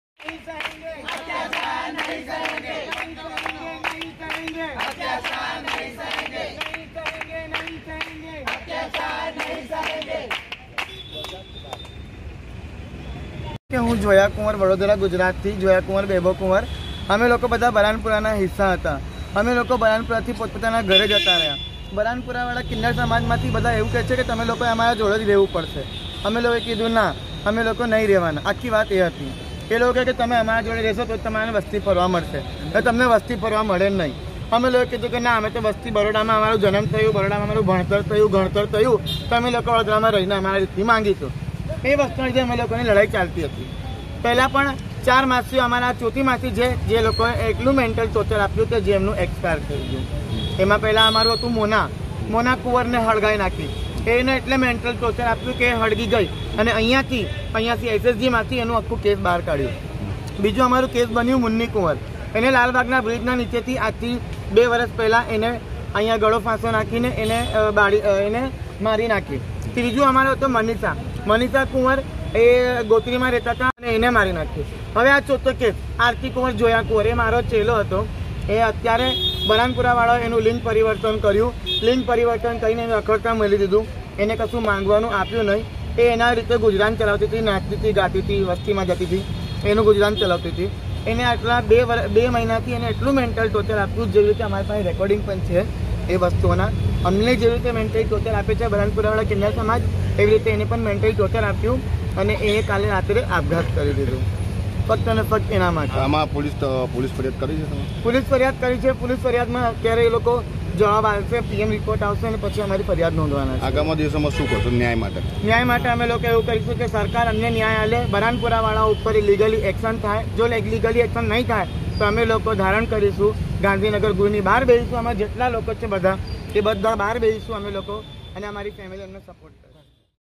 Yatharth News
વડોદરા ના કિશનવાડી વુડા ના મકાન માં રહેતા કિન્નર આકાશ ઉર્ફે આરતી એ ગળે ફાંસો ખાઈ ને કરી આત્મહત્યા..! આત્મહત્યા નું રહસ્ય અકબંધ..!